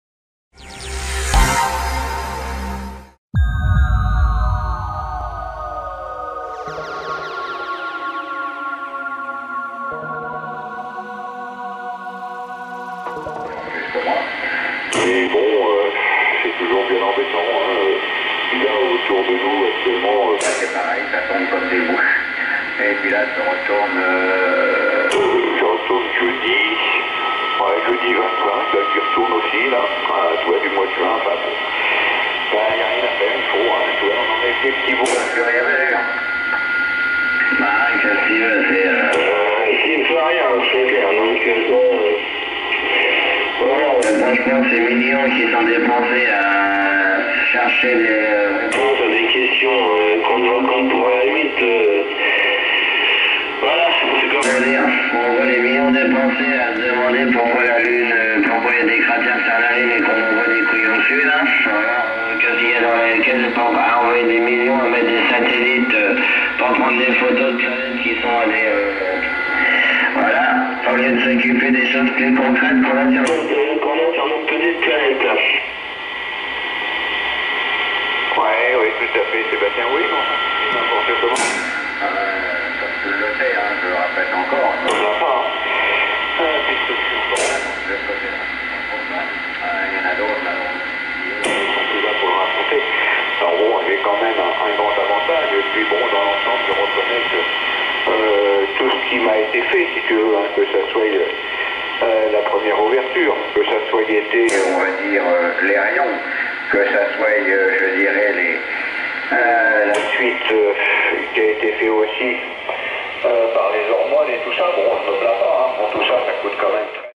Alan 555 vs Icom 740 - receivers test
Alan 555 is the most powerful CB radio from the nineties . Icom 740 is a classic multi-band transceiver , valued by operators. It was produced in 1982 .